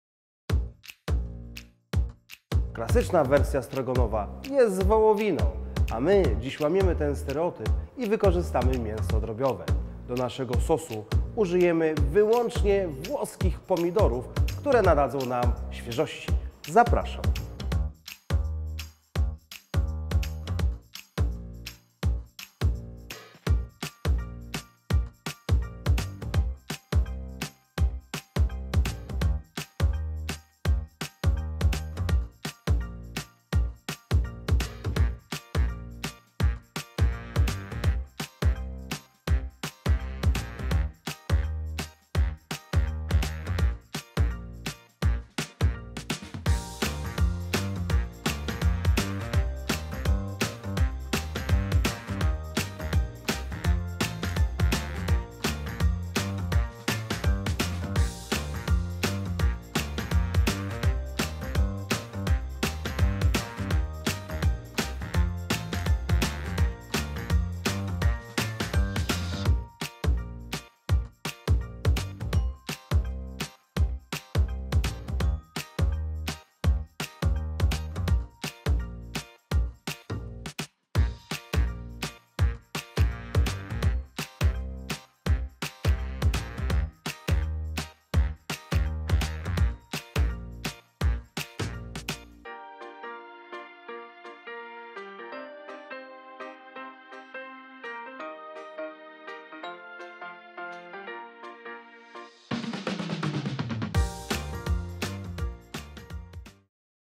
Przepis na strogonowa drobiowego - Mutti
Łamiemy stereotypy, wypróbuj przepis na strogonowa drobiowego z włoskimi pomidorami. Smacznego!